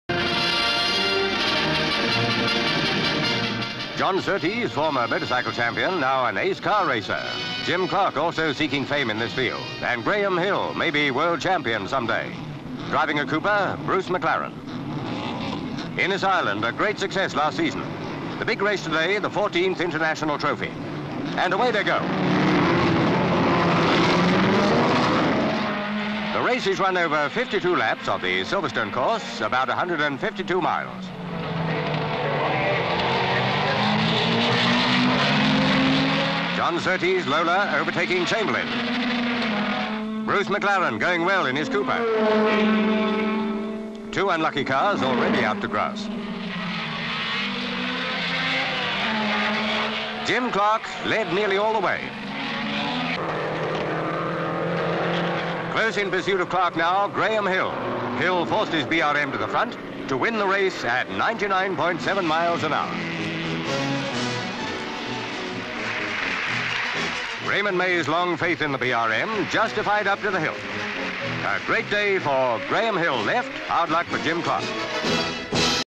Graham Hill Triumphs Aka Silverstone 14th International Trophy Race Won By Graham Hill In Brm (1962)
Title reads: "Graham Hill Triumphs".

Silverstone 14th International Trophy Race, Northamptonshire.

C/U driver John Surtees before the race.  M/S Jim Clark getting into his Lotus car. M/S Graham Hill gets into new V8 BRM car.  C/U Bruce McLaren driving a Cooper.  C/U Innes Ireland in his Ferrari.  C/U couple in crowd.

M/S cars lined up for the start. Cars shoot forward, pan as they go away from camera. M/S as leaders come into the first bend Copse Corner.  L/S as field sweeps round Copse Corner.  C/U of men in crowd. M/S cars racing down straight.  Panning shot Surtees overtaking Jay Chamberlain in Lotus.  Pan as Bruce McLaren in Cooper speeds along the straight. L/S another car coming towards camera, pans off to show two crashed cars lying beside the track.  C/U crashed car with right rear wheel almost off (car is Roy Salvadori's Lola), pan to the other car (Tony Shelly's Lotus).  Panning shot leader Clark coming out of bend and into straight.  Panning shot Clark at speed along straight, close behind is Graham Hill in the BRM.  Crowd.  Panning L/S as winning cars pass chequered flag.

L/S crowd applauding.  C/U Raymond Mays of BRM being congratulated by friends.  Crowd gathered round Hill and Clark on track.  C/U Graham Hill and Jim Clark.

Note: newspaper cutting and programme / press release on file.
 FILM ID:1703.32